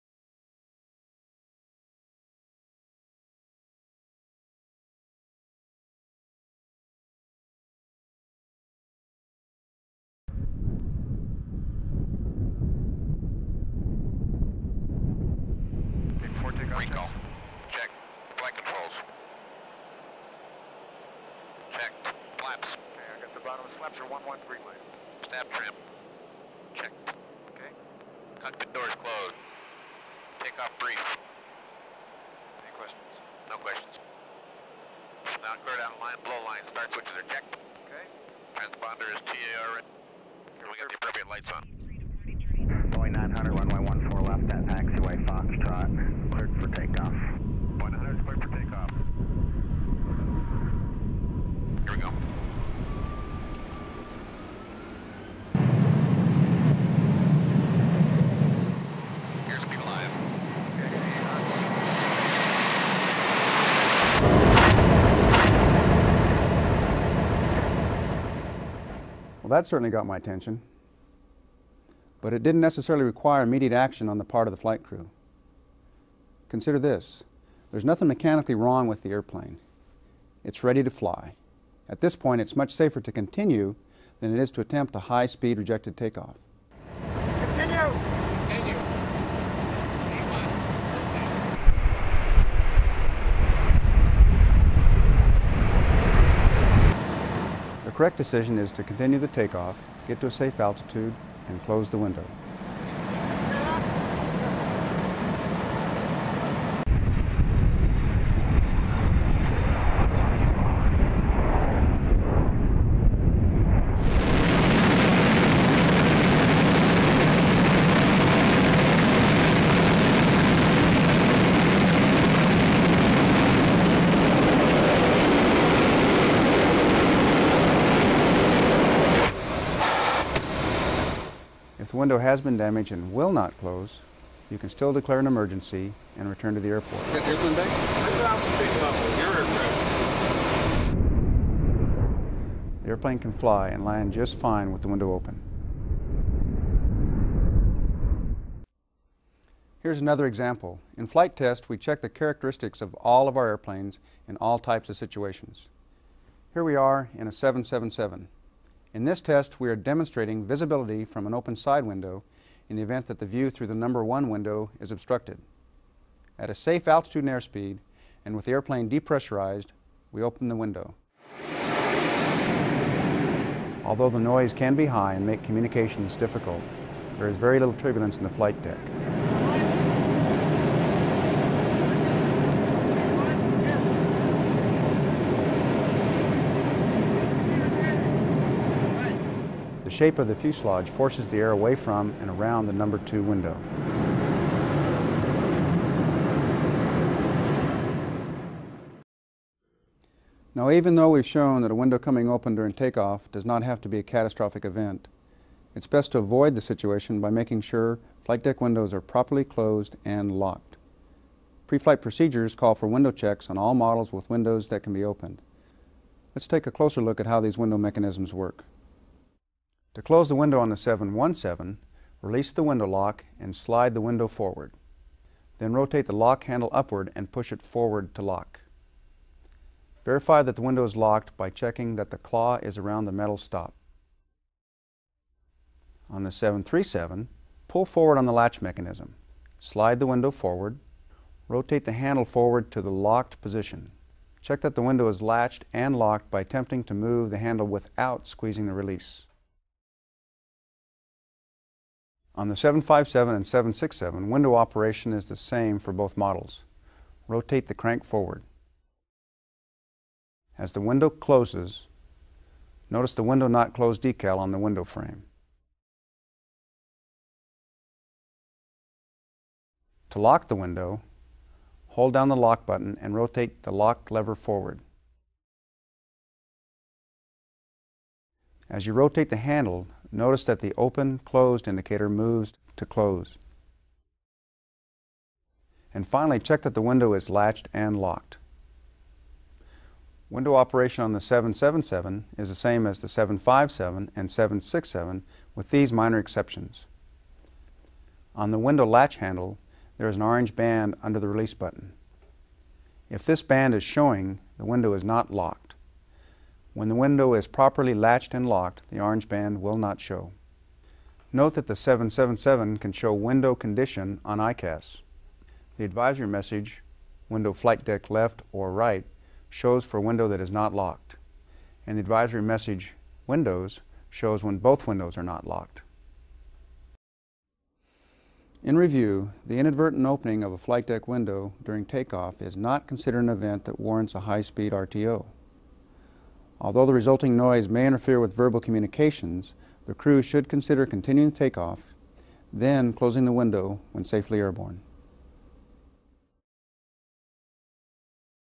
What to do when the window opens during take off? (ORIGINAL BOEING EDIT)
This is the unedited pilot tutorial from Boeing, from my previous video, "Can you fly a Boeing 737 with the window open?".